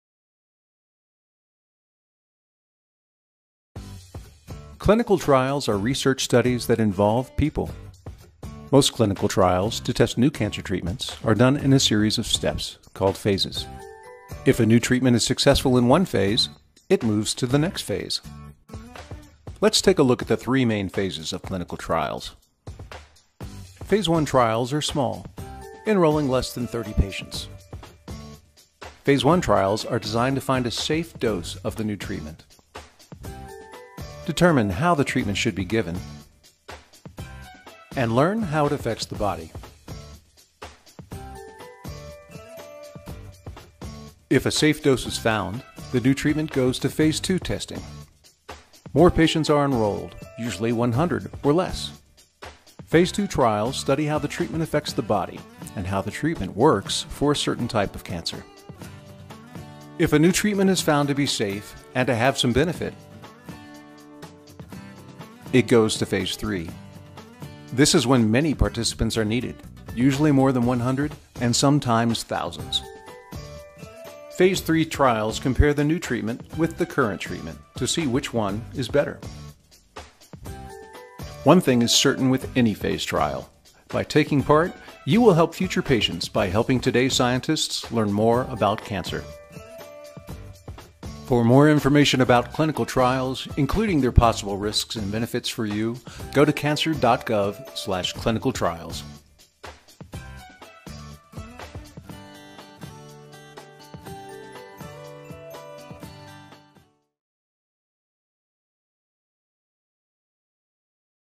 臨床試験の“相”とは？／米国国立がん研究所（NCI）
米国国立がん研究所（NCI）が制作した動画に、一社）日本癌医療翻訳アソシエイツ（JAMT/ジャムティ）が日本語字幕を付けたものです。

　ホワイトボード上の動画は、臨床試験の3つの主要な相について説明しています。臨床試験は研究を目的とした試験であり、人を対象に行われます。臨床試験はどんなものかを理解することは、皆さんや家族にとっての選択肢の1つとして臨床試験に参加するかどうかを判断するのに役立つでしょう。米国国立がん研究所が発信する臨床試験の詳細については以下のURLをご参照ください。

翻訳：ジャムティ翻訳チーム　字幕指導：瀧ノ島ルナ
監修：吉松由貴（飯塚病院　呼吸器内科）
原語動画公開日：2016/02/10
日本語版公開日: 2016/11/1